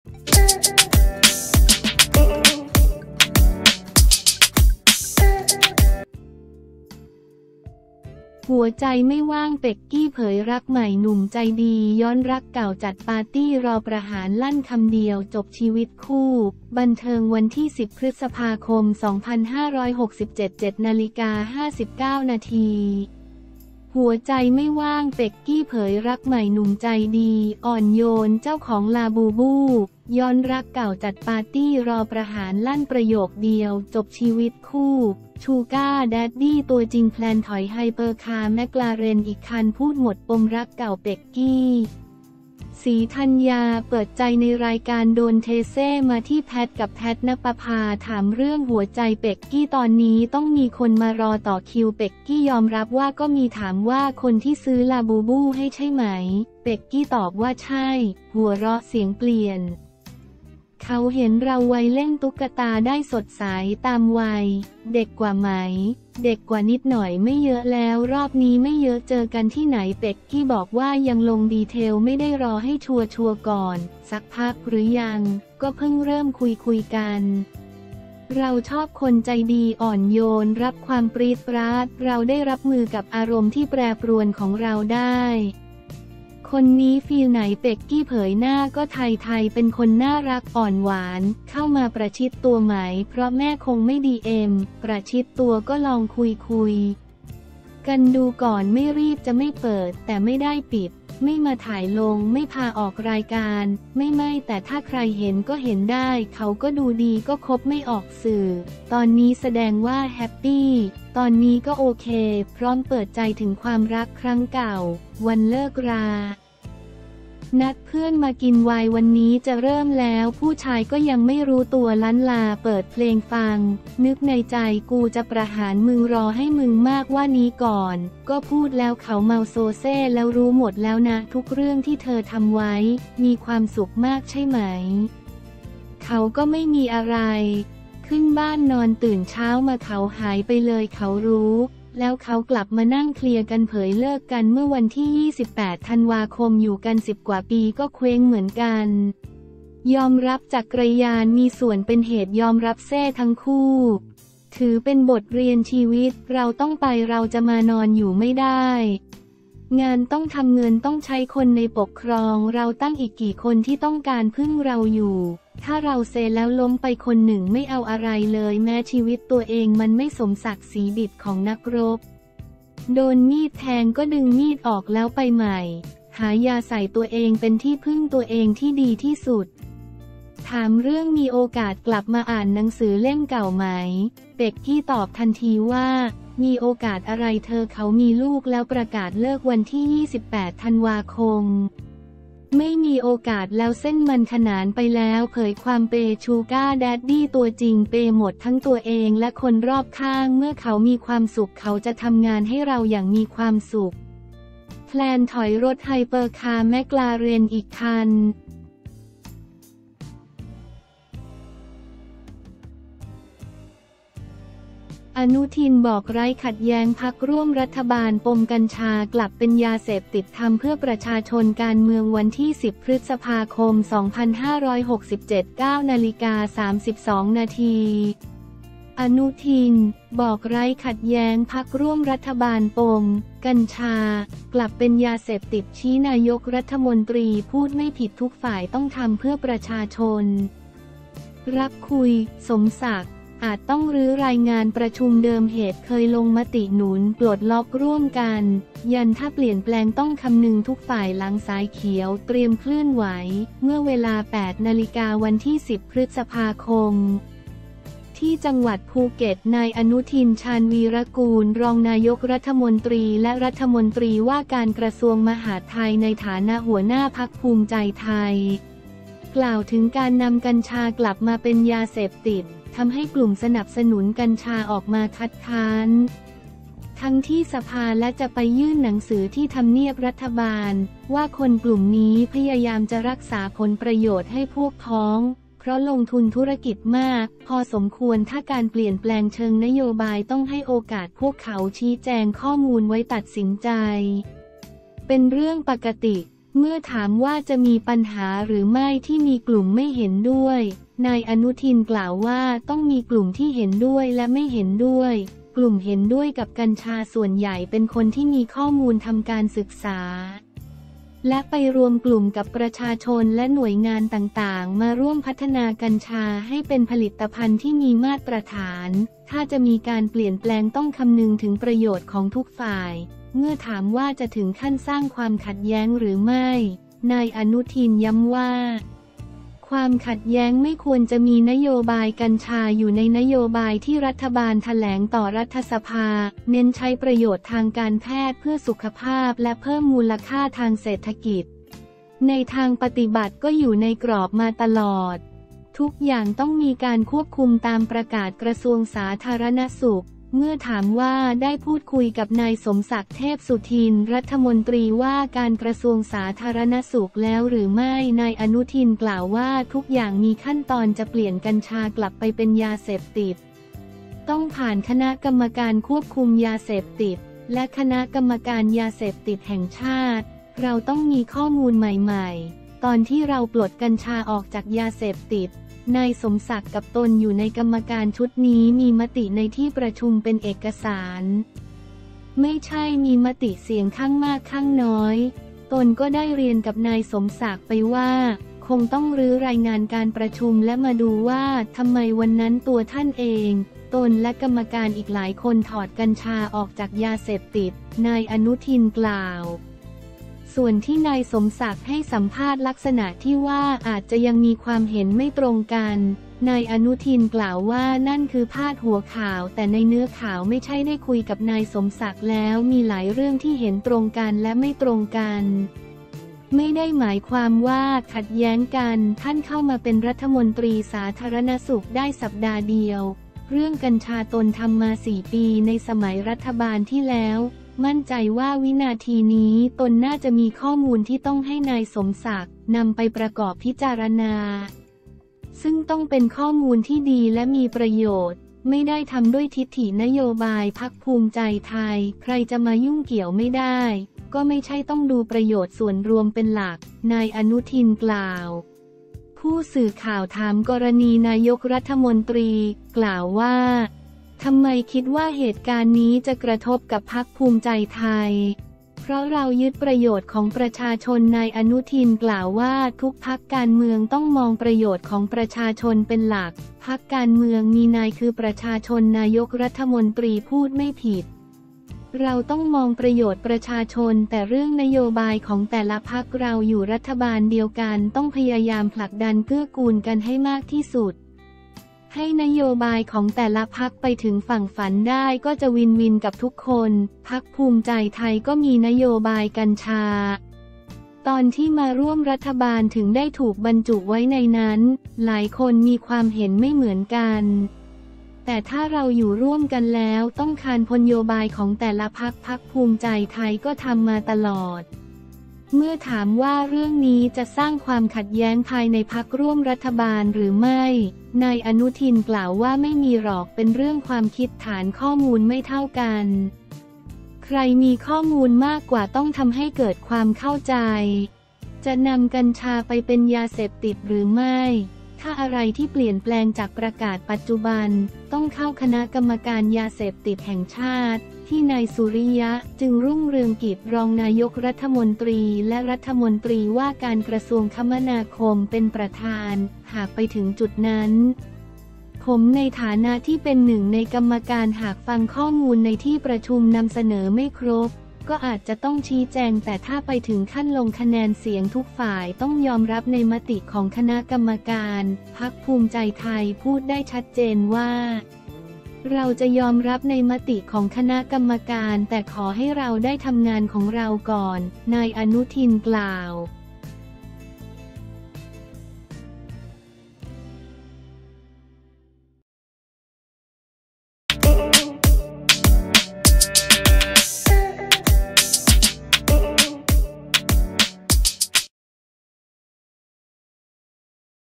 หัวใจไม่ว่าง เป็กกี้ เผยรักใหม่หนุ่มใจดี ย้อนรักเก่าจัดปาร์ตี้รอประหารลั่นคำเดียวจบชีวิตคู่
10 พ.ค. 2567
07:59 น.
หัวใจไม่ว่าง เป็กกี้ เผยรักใหม่หนุ่มใจดีอ่อนโยนเจ้าของลาบูบู้ ย้อนรักเก่าจัดปาร์ตี้รอประหารลั่นประโยคเดียวจบชีวิตคู่ ชูก้าร์แดดดี้ตัวจริง แพลนถอยไฮเปอร์คาร์ McLaren อีกคัน

พูดหมดปมรักเก่า เป็กกี้ ศรีธัญญา เปิดใจในรายการ โดนเทเซมาที่แพท กับ แพท ณปภา ถามเรื่องหัวใจ เป็กกี้ ตอนนี้ต้องมีคนมารอต่อคิว เป็กกี้ ยอมรับว่า ก็มี
ถามว่าคนที่ซื้อลาบูบู้ให้ใช่ไหม เป็กกี้ ตอบว่า ใช่ (หัวเราะเสียงเปลี่ยน) เขาเห็นเราวัยเล่นตุ๊กตาได้ สดใสตามวัย (เด็กกว่าไหม) เด็กกว่านิดหน่อย ไม่เยอะแล้วรอบนี้ไม่เยอะ

เจอกันที่ไหน เป็กกี้ บอกว่า ยังลงดีเทลไม่ได้ รอให้ชัวร์ๆ ก่อน (สักพักหรือยัง) ก็เพิ่งเริ่มคุยๆกัน เราชอบคนใจดี อ่อนโยน รับความปรี๊ดปร๊าดเราได้ รับมือกับอารมณ์ที่แปรปรวนของเราได้
คนนี้ฟีลไหน เป็กกี้ เผยหน้าก็ไทยๆ เป็นคนน่ารักอ่อนหวาน (เข้ามาประชิดตัวไหม เพราะแม่คงไม่ DM) ประชิดตัว ก็ลองคุยๆ กันดูก่อน ไม่รีบ จะไม่เปิด แต่ไม่ได้ปิด (ไม่มาถ่าย ลง ไม่พาออกรายการ) ไม่ๆ แต่ถ้าใครเห็นก็เห็นได้ เขาก็ดูดี ก็คบไม่ออกสื่อ (ตอนนี้แสดงว่าแฮปปี้) ตอนนี้ก็โอเค
พร้อมเปิดใจถึงความรักครั้งเก่า วันเลิกรา “นัดเพื่อนมากินไวน์ วันนี้จะเริ่มแล้ว ผู้ชายก็ยังไม่รู้ตัวลั้ลลาเปิดเพลงฟัง นึกในใจ กูจะประหารมึง รอให้มึงเมากว่านี้ก่อน
“ก็พูด แล้วเขาเมาโซเซแล้ว รู้หมดแล้วนะทุกเรื่องที่เธอทำไว้ มีความสุขมากใช่ไหม เขาก็ไม่มีอะไร ขึ้นบ้านนอน ตื่นเช้ามาเขาหายไปเลย เขารู้”

แล้วเขากลับมานั่งเคลียร์กัน เผยเลิกกันเมื่อ 28 ธันวาคม อยู่กัน 10 กว่าปี ก็เคว้งเหมือนกัน ยอมรับจักรยานมีส่วนเป็นเหตุ ยอมรับเซทั้งคู่
ถือเป็นบทเรียน ชีวิตเราต้องไป เราจะมานอนอยู่ไม่ได้ งานต้องทำ เงินต้องใช้ คนในปกครองเราตั้งอีกกี่คนที่ต้องการพึ่งเราอยู่

“ถ้าเราเซแล้วล้มไปคนหนึ่ง ไม่เอาอะไรเลย แม้ชีวิตตัวเอง มันไม่สมศักศรีดิ์ของนักรบ โดนมีดแทงก็ดึงมีดออกแล้วไปใหม่ หายาใส่ ตัวเองเป็นที่พึ่งตัวเองที่ดีที่สุด
ถามเรื่องมีโอกาสกลับมาอ่านหนังสือเล่มเก่าไหม เป็กกี้ ตอบทันทีว่า “มีโอกาสอะไรเธอ เขามีลูกแล้ว ประกาศเลิก 28 ธันวาคม ไม่มีโอกาสแล้ว เส้นมันขนานไปแล้ว
เผยความเปย์ ชูก้าร์แดดดี้ตัวจริง เปย์หมดทั้งตัวเอง และคนรอบข้าง เมื่อเขามีความสุข เขาจะทำงานให้เราอย่างมีความสุข แพลนถอยรถไฮเปอร์คาร์ McLaren อีกคัน